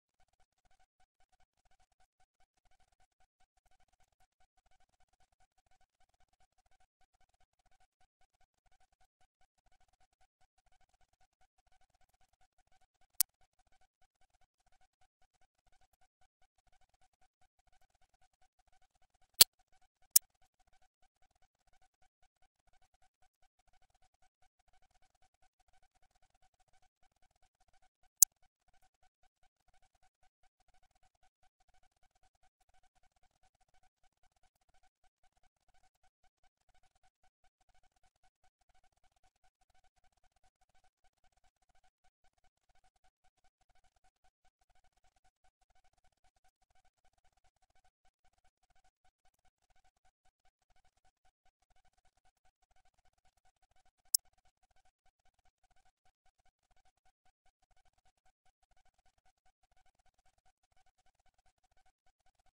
抜歯後治癒不良部のレーザーPoor healing after tooth extraction
抜歯後の治りが悪く、痛みがあります。炭酸ガスレーザー照射。